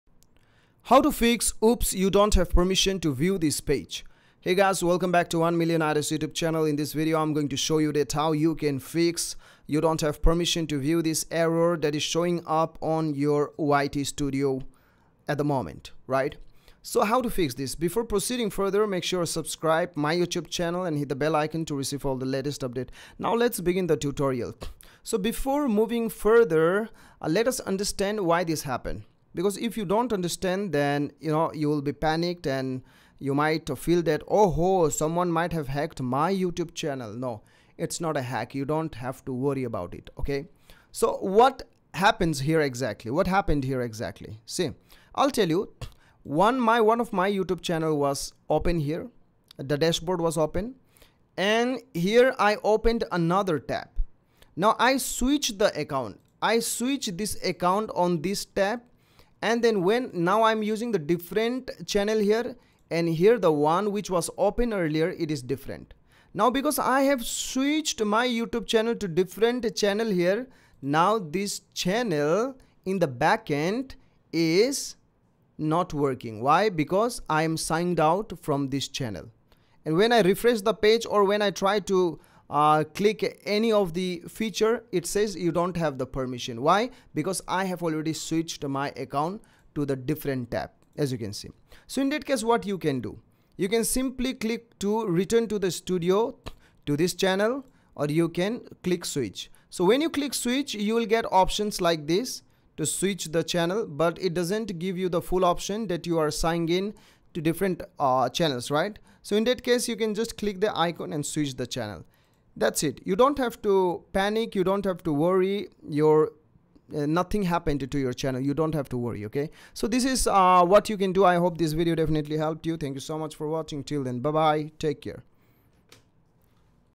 Oops you don't have permission to view this page Solved
If you're seeing the "Oops you don't have permission to view this page" error, don't worry! In this video, we'll show you how to solve this issue and access the content you're trying to view.